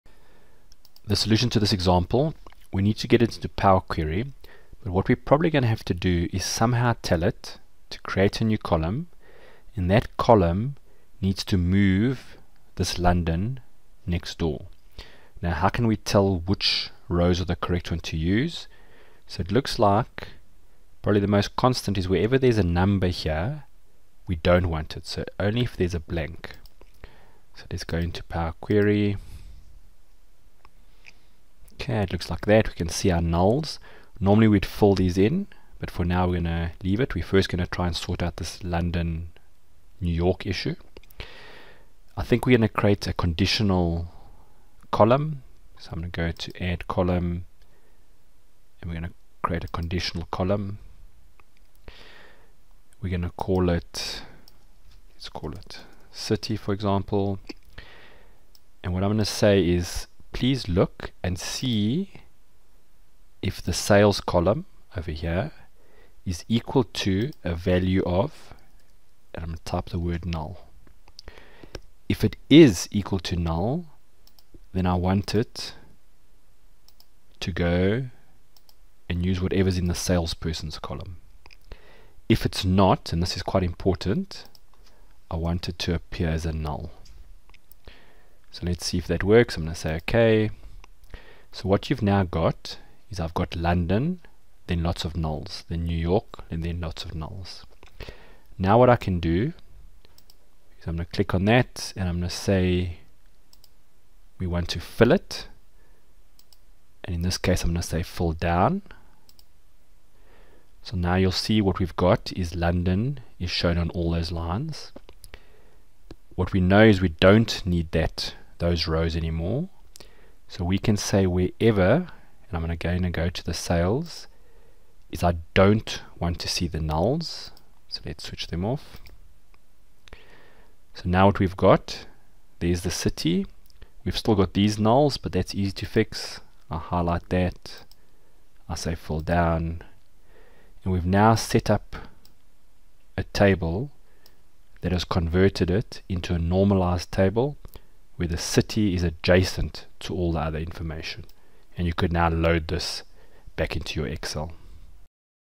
Turn a report into a flat file using Power Query
00:00 Make a flat file database where sub headings populate in a separate column
00:28 Import the table with gaps and sub headings into Power Query
00:46 Create a column which brings only the sub headings into it
01:00 IF row is a sub heading, show the sub heading otherwise null
01:39 Fill in gaps of the sub heading so it appears on every row
02:00 Remove the sub heading rows as we don't need them anymore
02:16 Fill in the other gaps so that you have a flat file

How to take a report (with important subheadings and gaps between data which needs to be dragged down) and convert it to a flat file where all the key data is in its own columns and on every row WITHOUT manually dragging down the cells. This uses Power Query